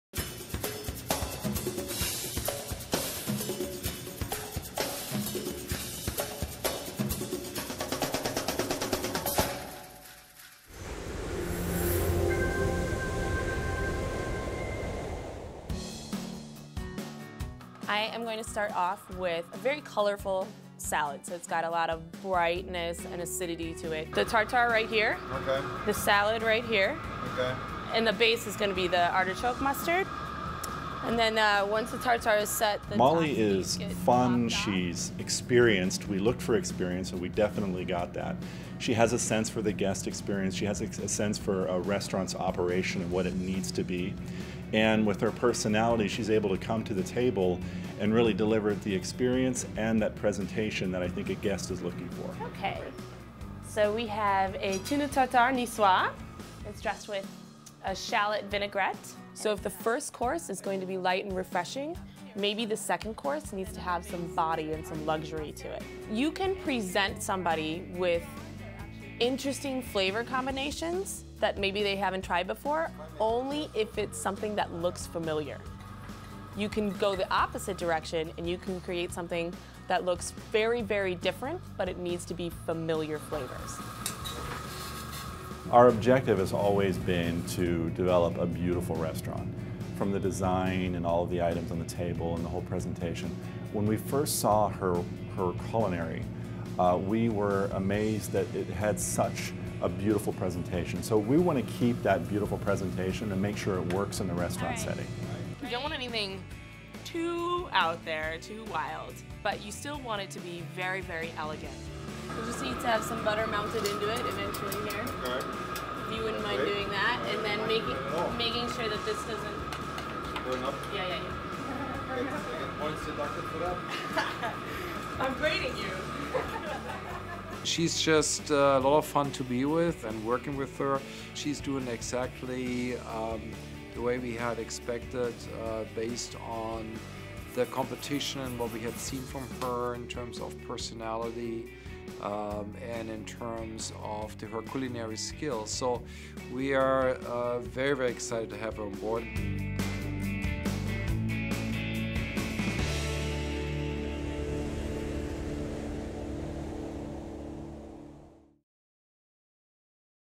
A look at the kinds of food a guest on board the Allure of the Seas can expect
A look at the kinds of food a guest on board the Allure of the Seas can expect at the '150 Central park' restaurant